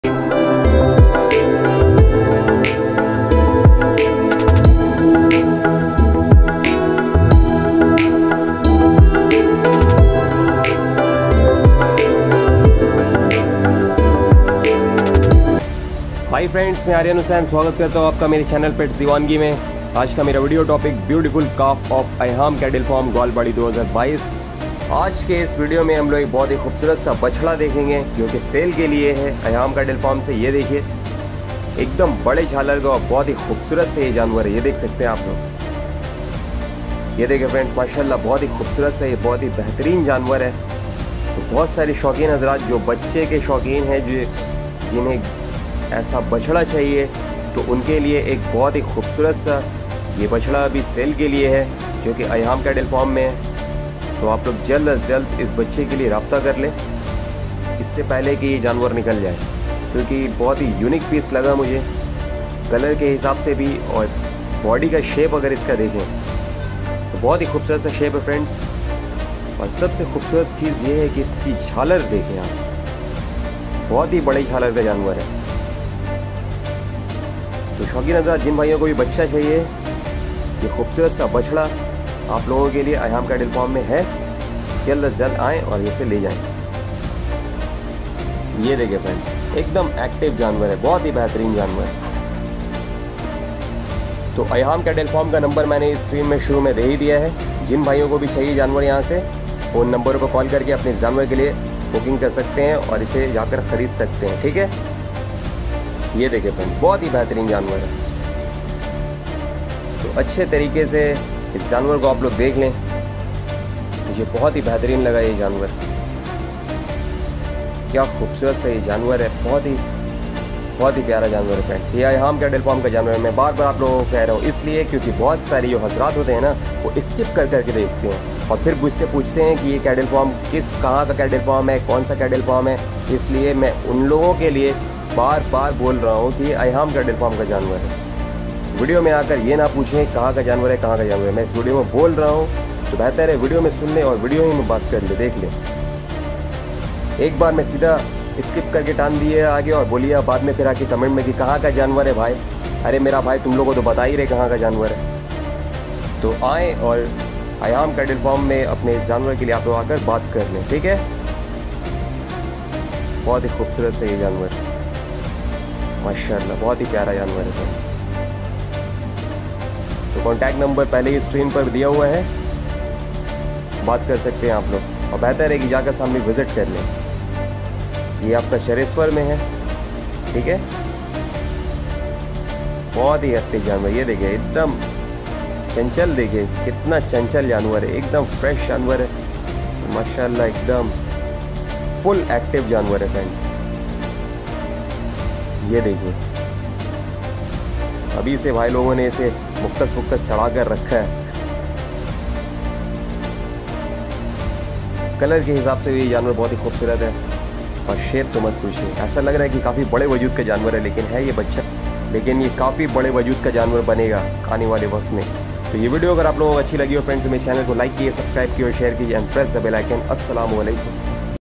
Ayham Cattle farm beautiful baby cow | kolkata cow 2022 |Gowalbari 2022
Hi friends in this video I am going to show you the most beautiful calf of Gowalbari from Ayham Cattle farm gowalbari @TheHomeofGoruLovers